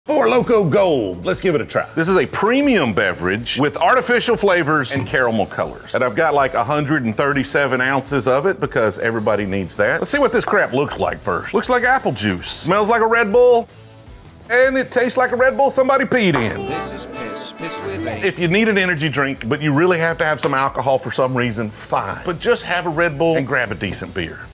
This Old Man Tries Four Loko Gold
Looking for an honest and entertaining review of Four Loko Gold? Look no further! In this video, we'll be taking a closer look at Four Loko Gold, a popular malt beverage that's been making waves in the alcohol world. We'll be discussing everything from its appearance, aroma, and taste, to its overall potency and effects.